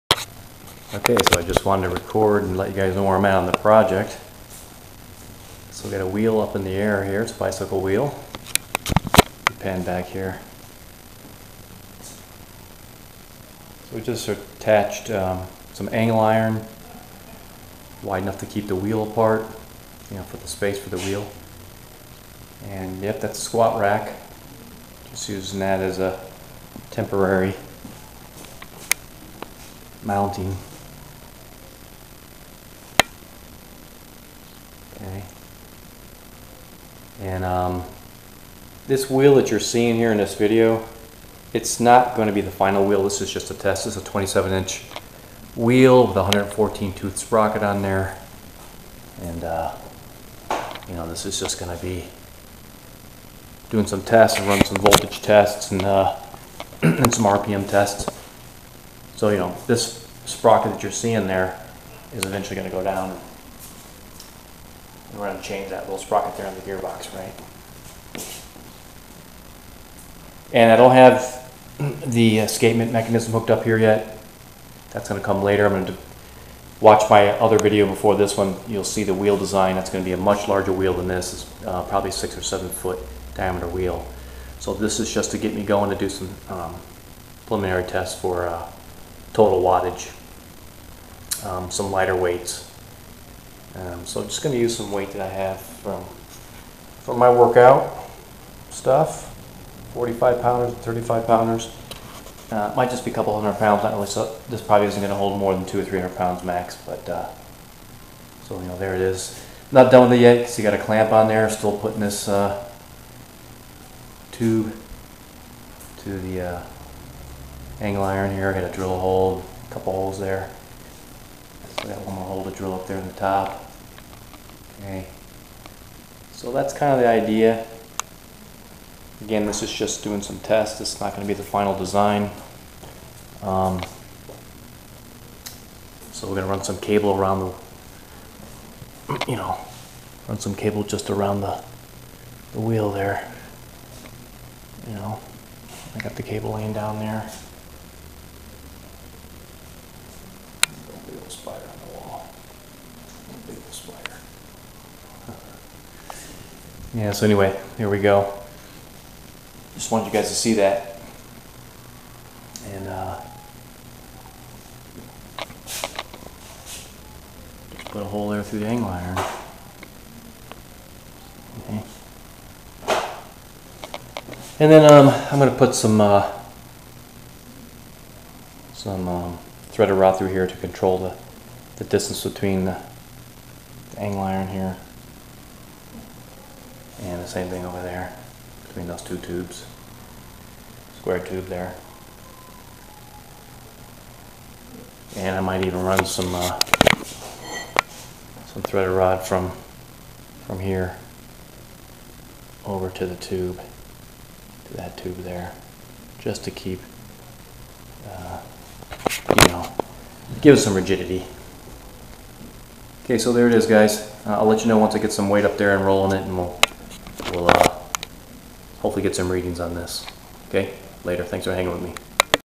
45 - Winding Barrel (Wheel) 27 inch
A small 27 inch diameter winding wheel/barrel to do some wattage and RPM/Torque testing to the PMA.